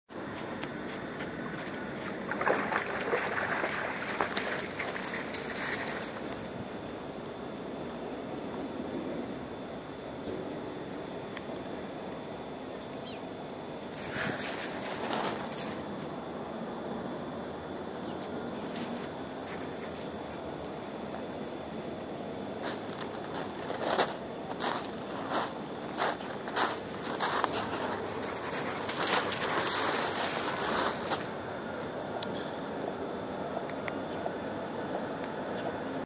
RAIKO'S MORNING SWIM
Alaskan Malamute Raiko swims in Greek waters.